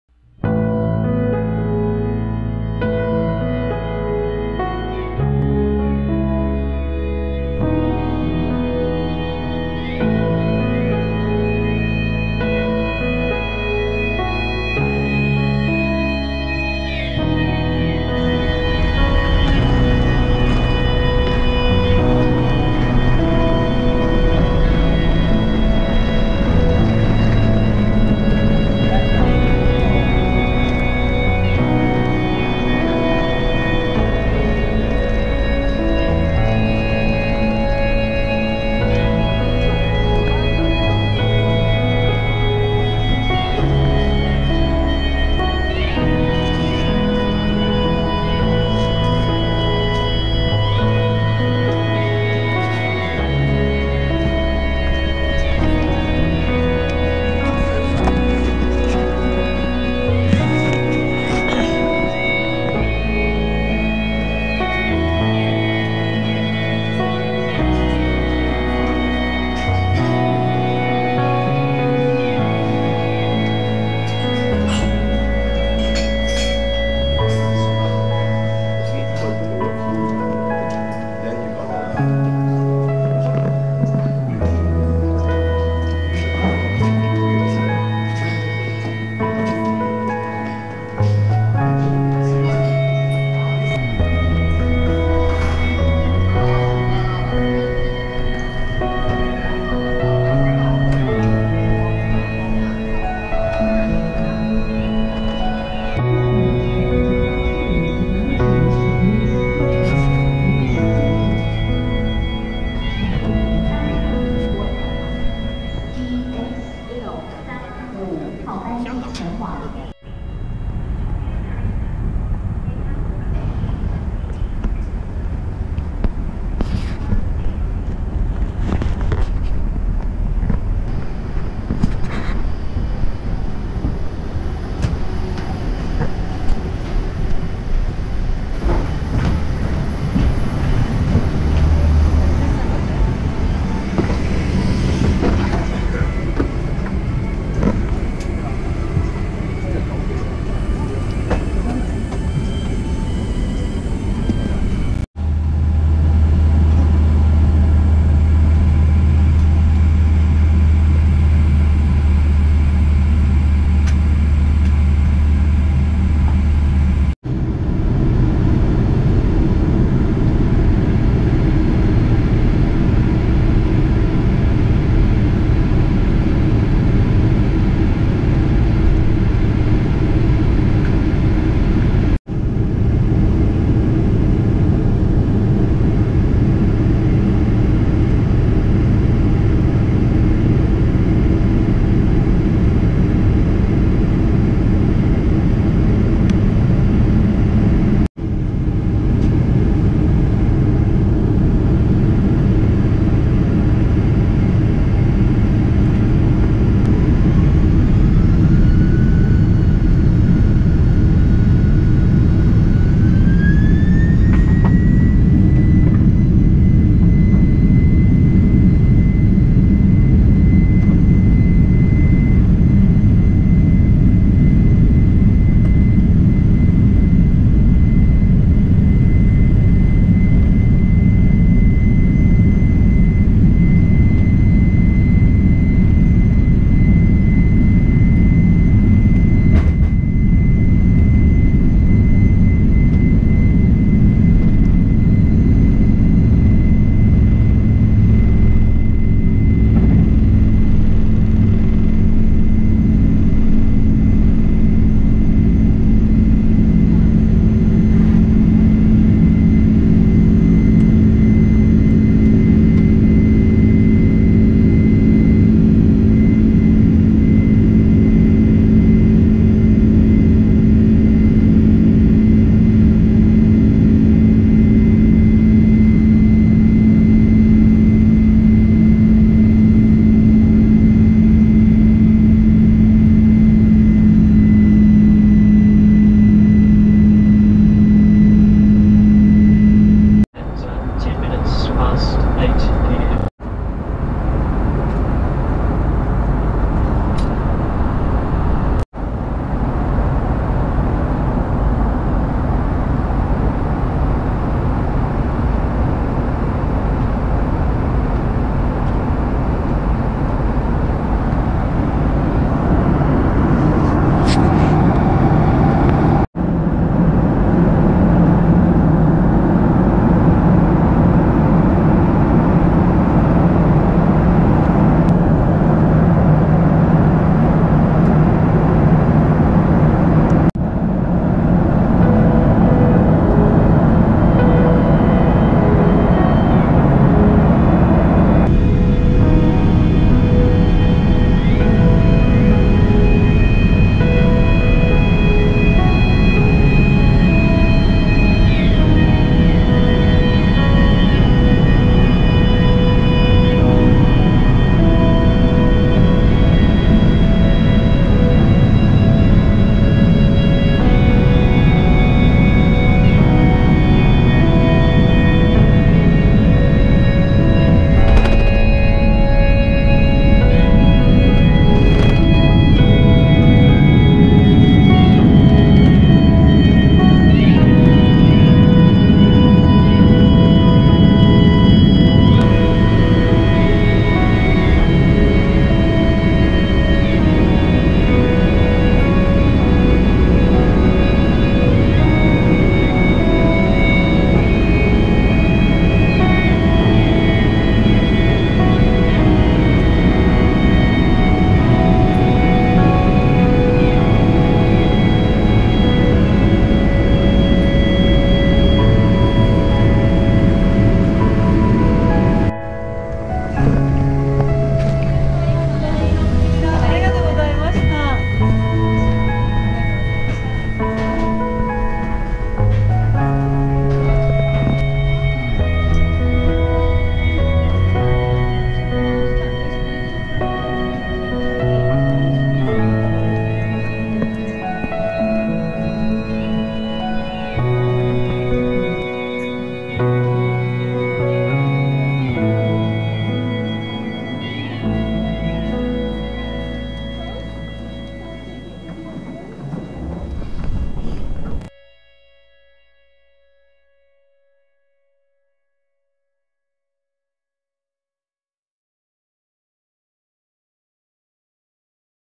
Air Japan (ANA) 767-300 Business Class HKG-NRT, Round the World 2-6
Round the World 2
LAX-MUC-FRA-DEL-BKK-HKG-NRT-SFO-BUR
Flight Miles: 20,158
ANA Award: 180,000 Miles + SFO-BUR